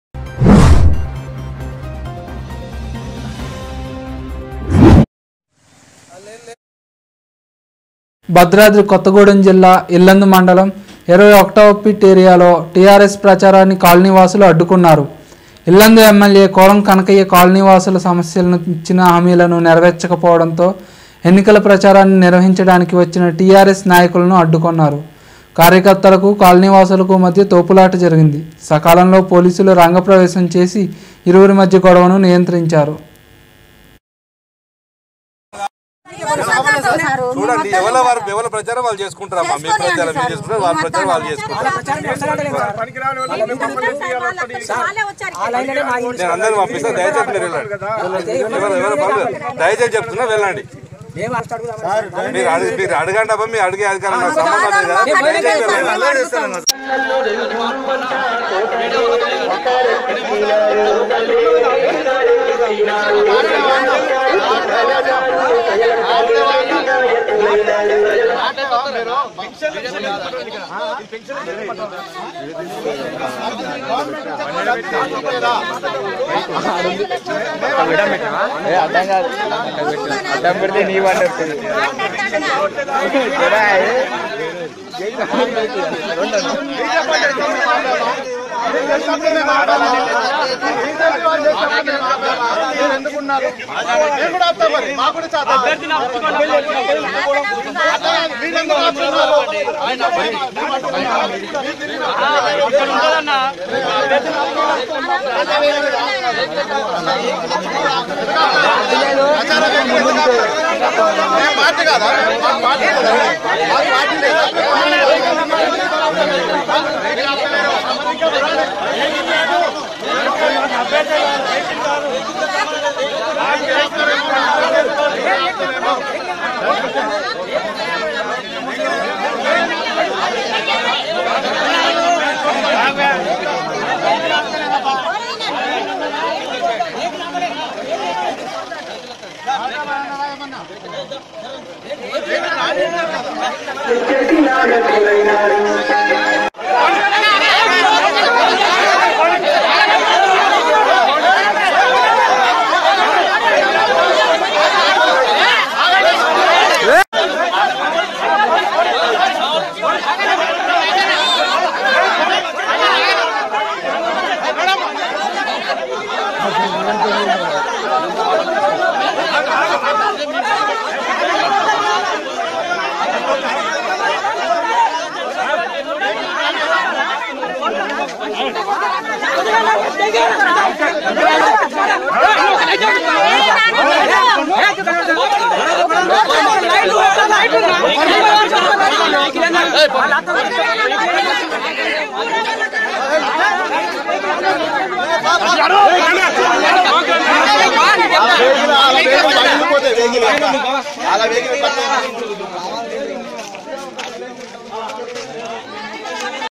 ఎన్నికల ప్రచారాన్ని అడ్డుకున్న కాలనీ వాసులు: భద్రాద్రి కొత్తగూడెం జిల్లా
భద్రాద్రి కొత్తగూడెం జిల్లా ఇల్లందు మండలం 21వ పిట్ ఏరియాలో టిఆర్ఎస్ ప్రాచారాన్ని కాలనీ వాసులు అడ్డుకు న్నారు. ఇల్లందు ఎమ్మెల్యే కోరం కనకయ్య కాలనీ వాసుల సమస్యల ను, ఇచ్చిన హామీలను నెరవేర్చక పోవడంతో ఎన్నికల ప్రచారాన్ని నిర్వహించడానికి వచ్చిన టిఆర్ఎస్ నాయకులను అడ్డుకొన్నరు. కార్యకర్తలకు, కాలనీ వాసులకు మధ్య తోపులాట జరిగింది. సకాలంలో పోలీసులు రంగ ప్రవేశం చేసి ఇరువురి మధ్య గొడవను నియంత్రించారు.